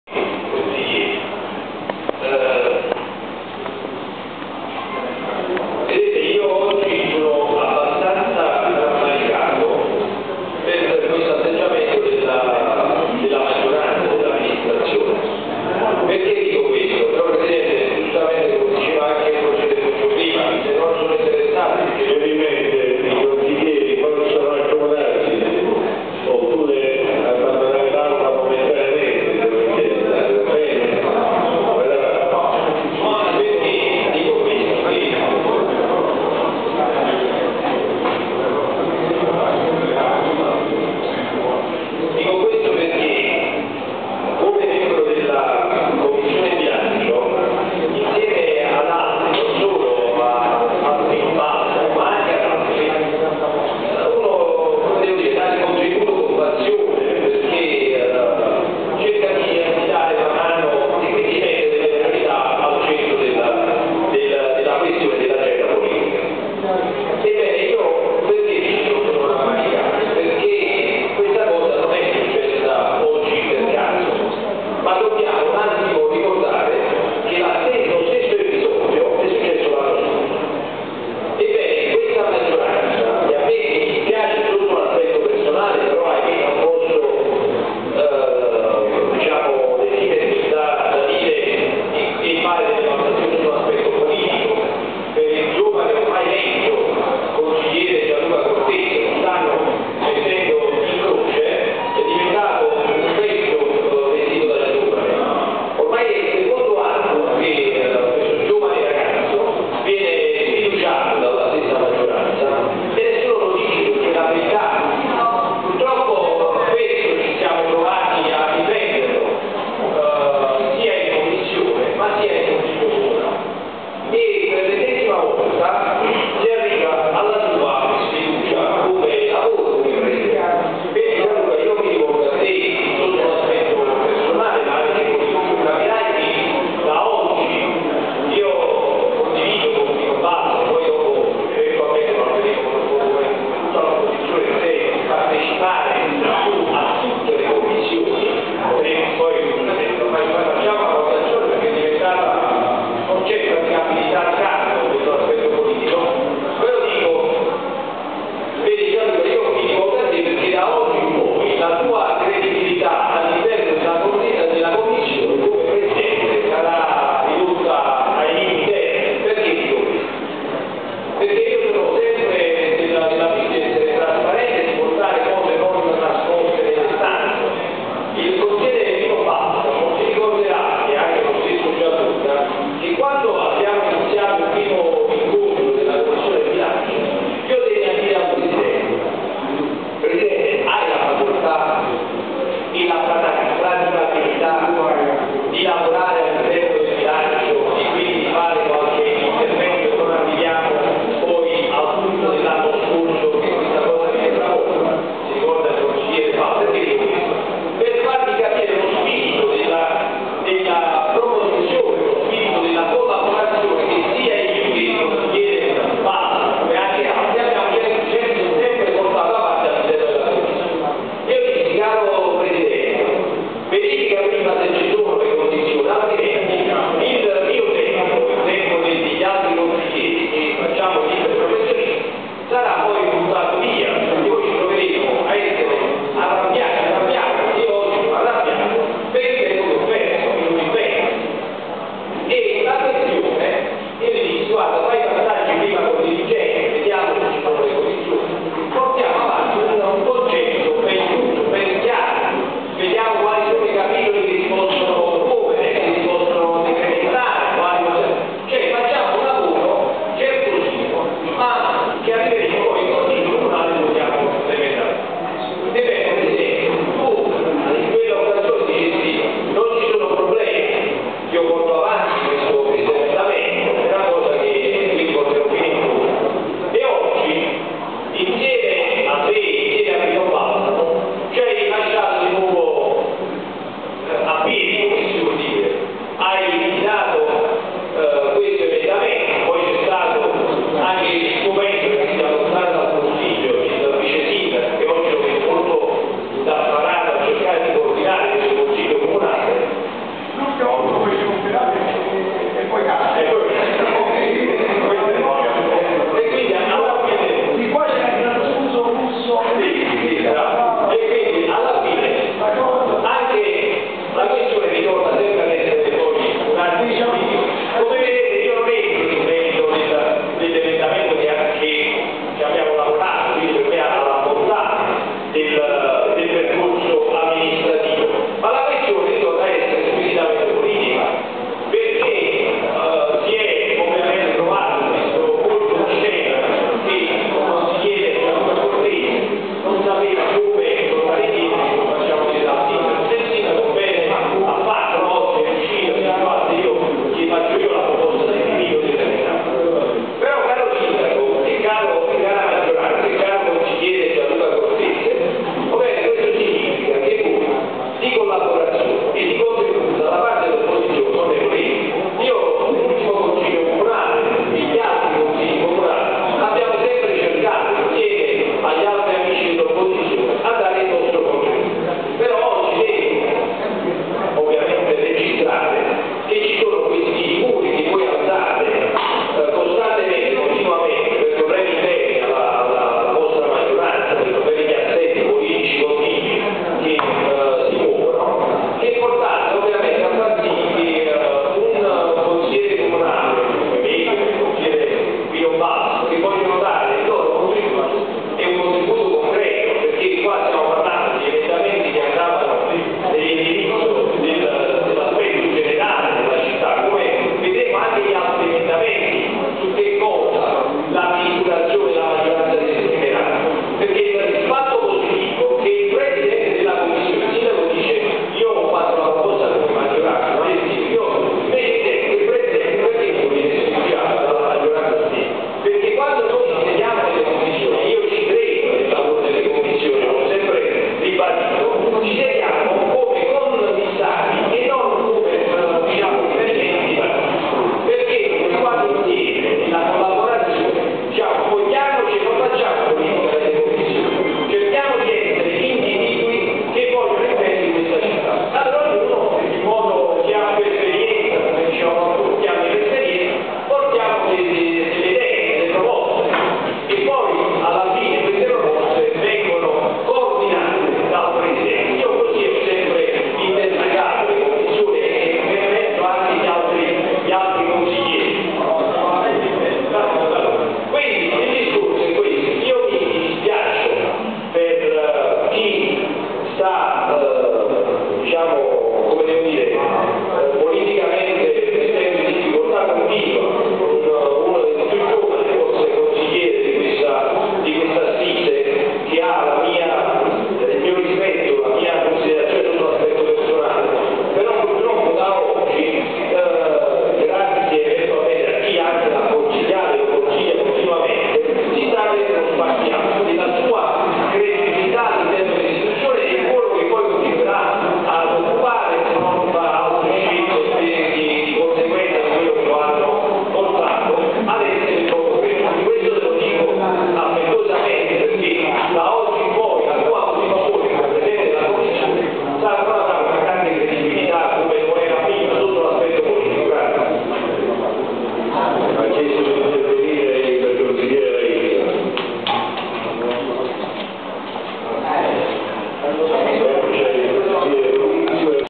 Casoria 10/10/2014 - Consiglio Comunale (21)
"Casoria Ieri & Oggi online" presenta:
Casoria 10 ottobre 2014, il consiglio comunale è convocato in seconda convocazione per il giorno 10  ottobre 2014 alle ore 09,00 presso la sala Consiliare della  Sede Comunale sita in P.za Cirillo per la trattazione dei seguenti argomenti:
1) ;
2) ;
3) ;
4) ;
5) .
Intervento del consigliere Massimo Iodice (?)